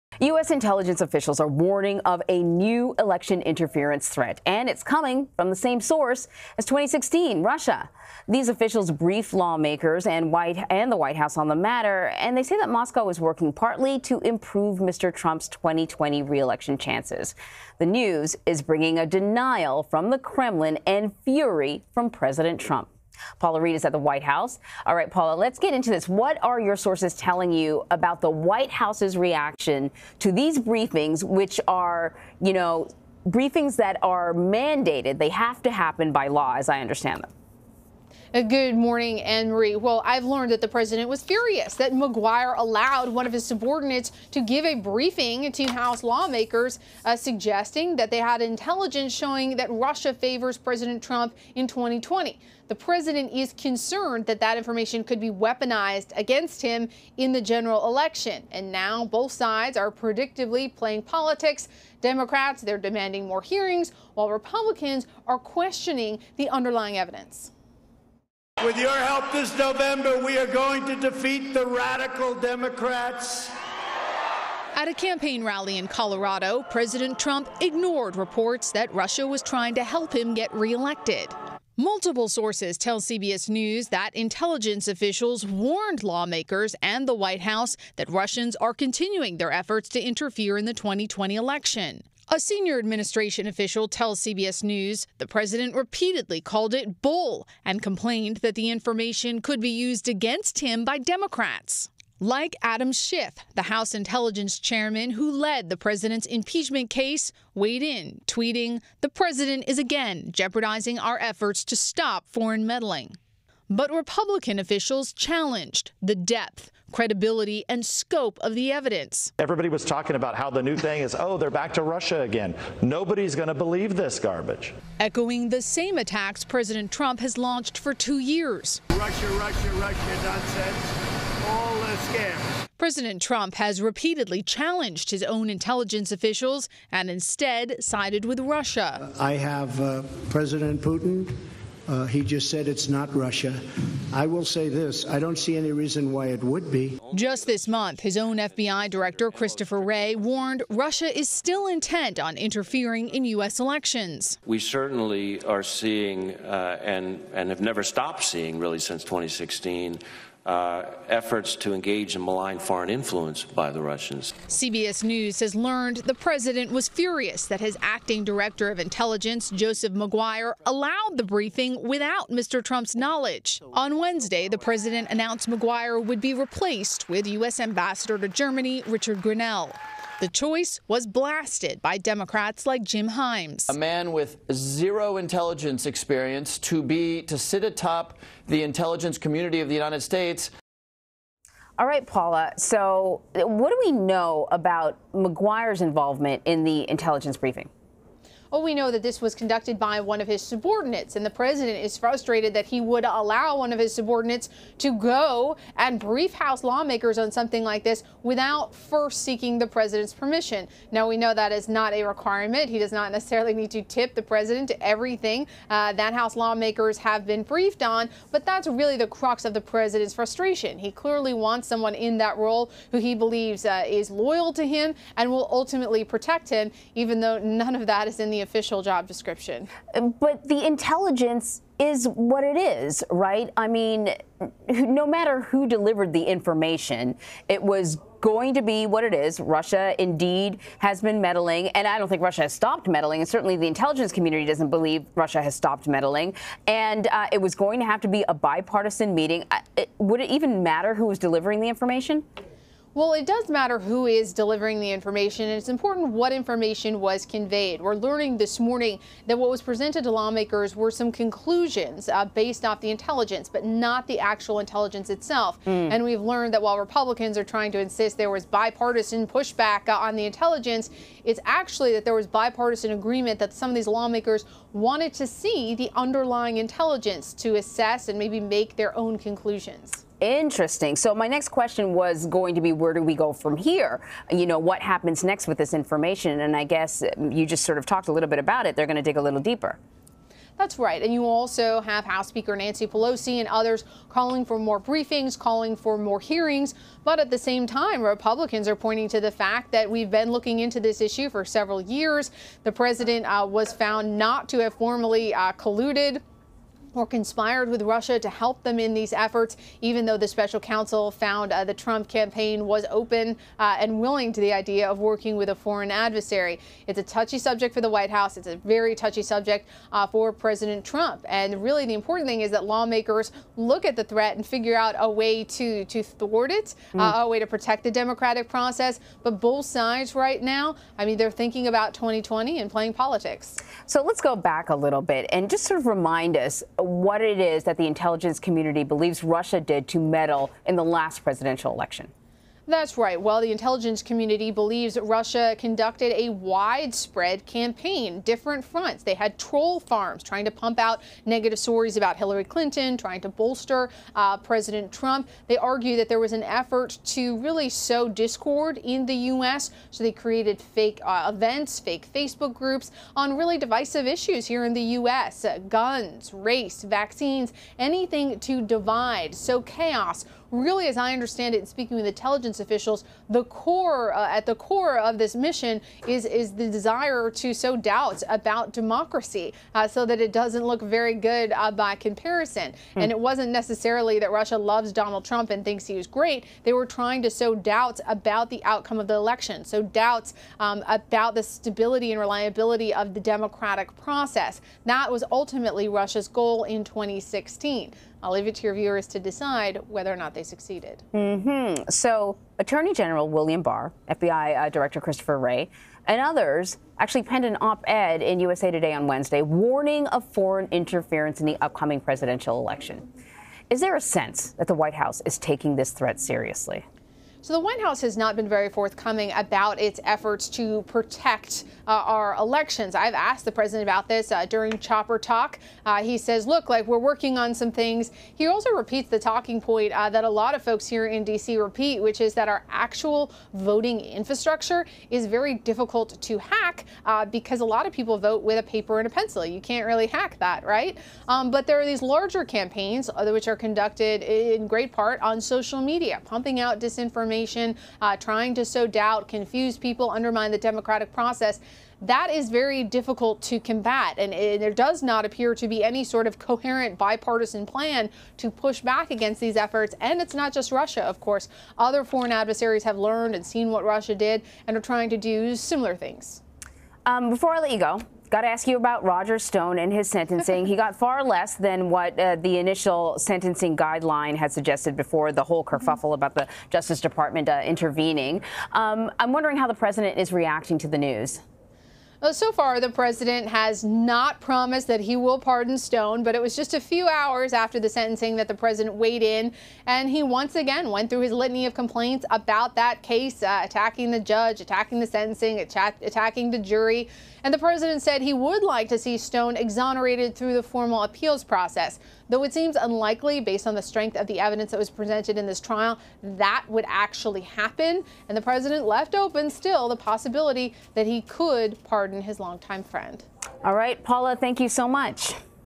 U.S. intelligence warns Russia is meddling in 2020 election in part to help reelect Trump
U.S. intelligence officials have told Congress and the White House that Russia is once again interfering in the upcoming U.S. presidential election, in part to boost President Trump's reelection chances. Russia denies this accusation. CBS News White House correspondent Paula Reid joined CBSN AM with the latest.